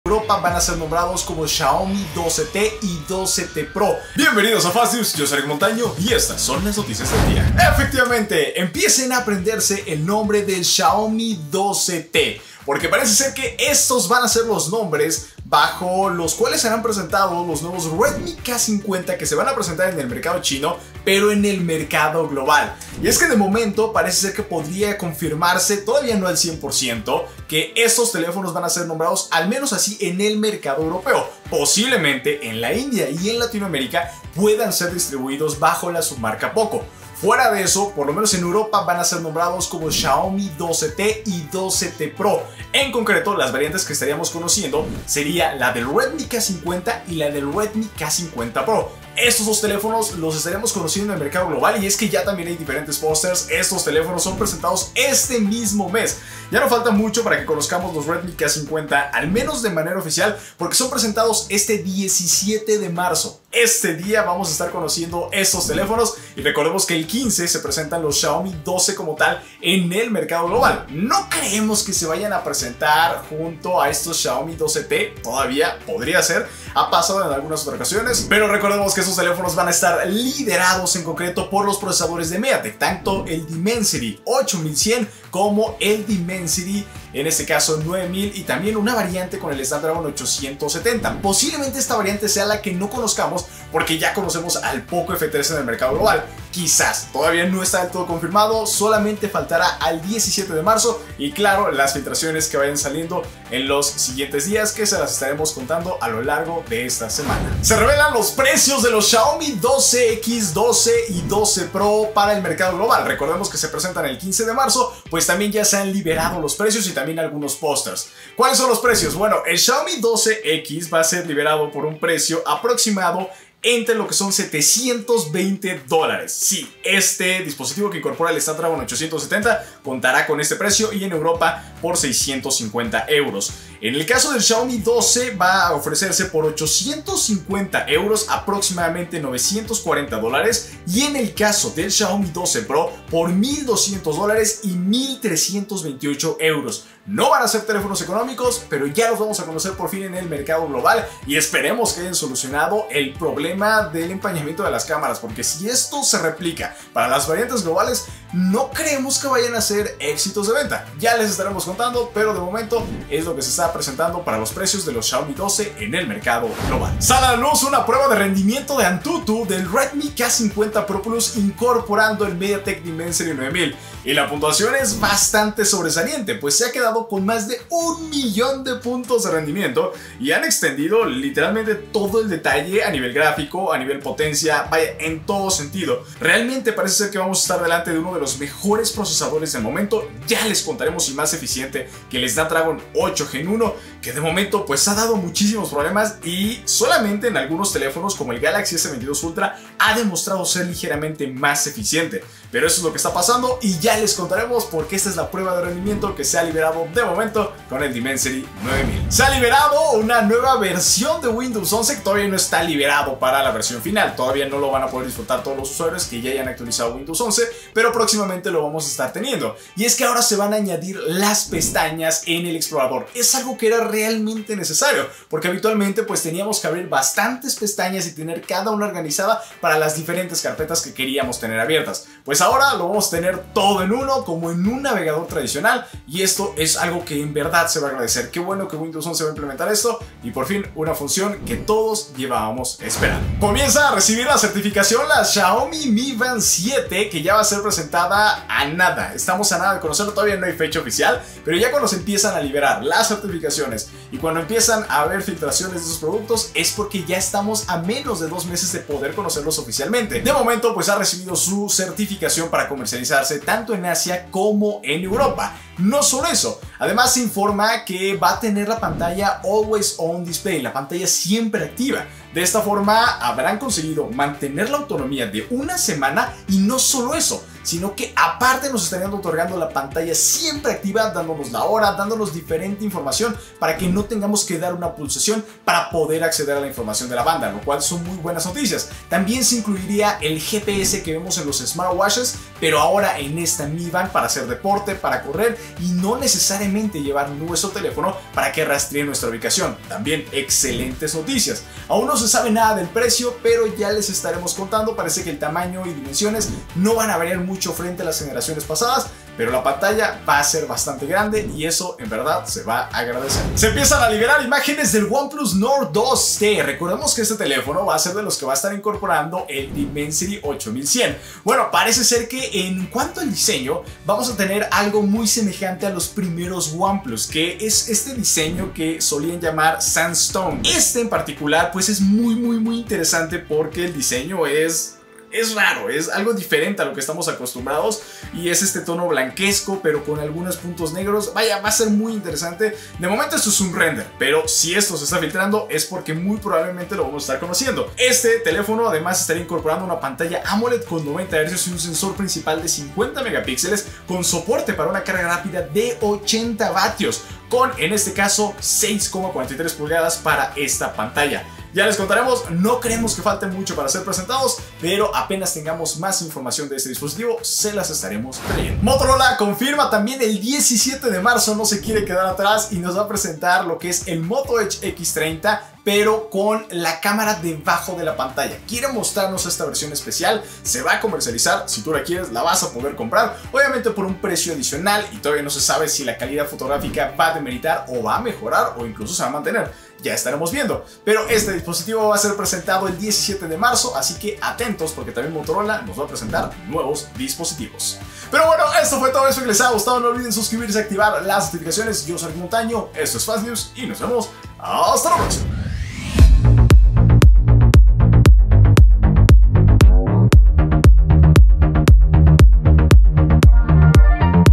Xiaomi 12T Pro serán los Redmi K50 Pro GLOBALES, ADIOS POCO F4!? | FASTNEWS
▶HOY TE MUESTRO EN FASTNEWS
00:00 Introducción
00:07 Xiaomi 12T y 12T Pro serán los Redmi K50 Globales
01:47 Precios globales del Xiaomi 12x, Xiaomi 12, Xiaomi 12 Pro
02:46 Redmi K50 Pro+ con Dimensity 9000 prueba de antutu
03:28 Llegan las ventanas al explorador de Windows 11
04:03 Filtraciones de la Xiaomi Mi Band 7
05:13 Oneplus Nord 2T traerá un diseño clásico
06:08 Moto Edge X30 con cámara bajo la pantalla